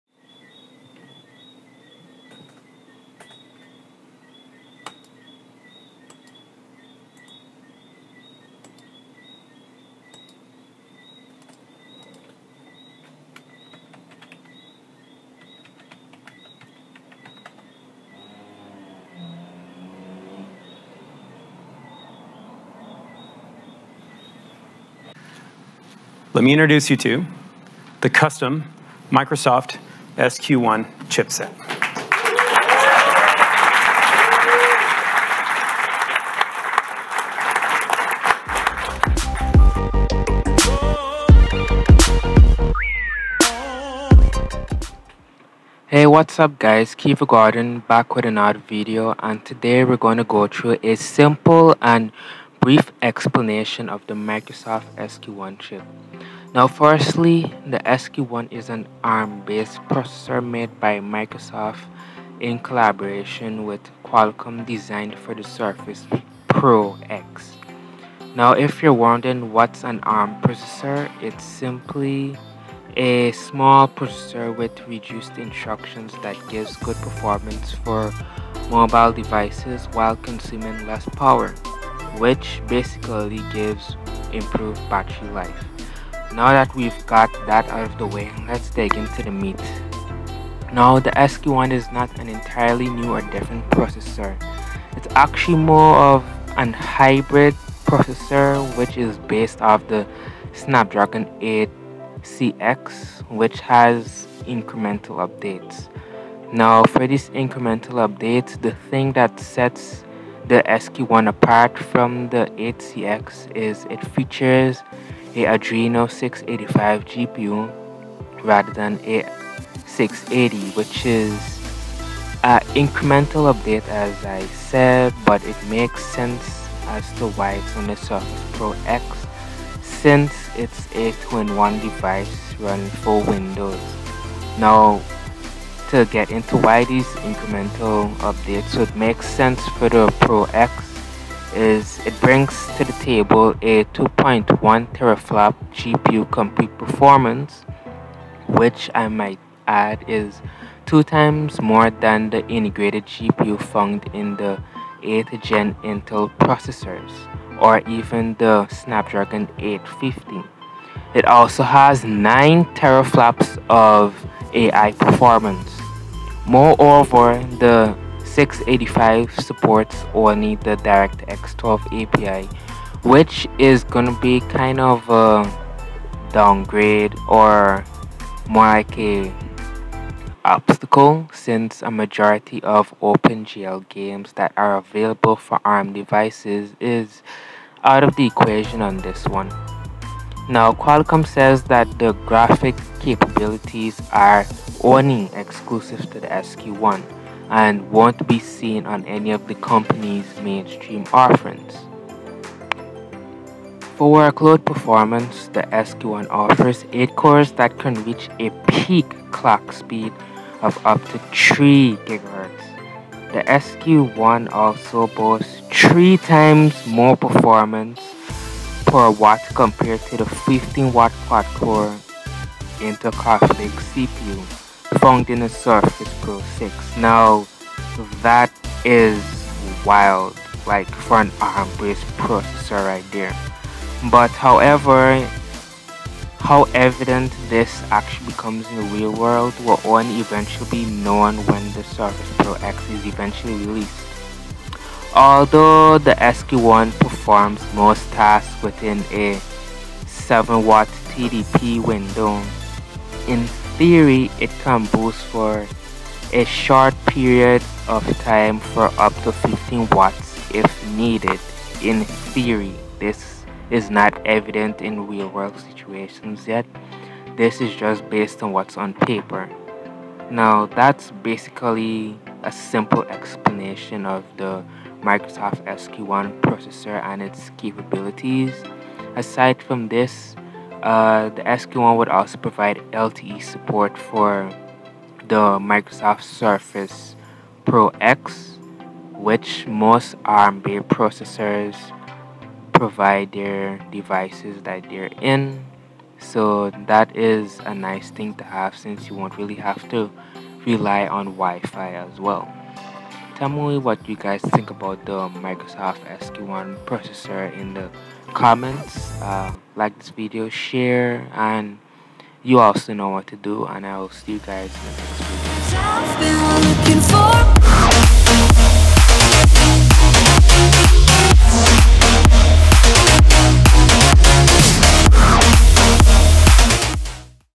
The Microsoft SQ1 Explained: In 5 Minutes!
A simple exponentiation of the SQ1 processor found on the Surface Pro X.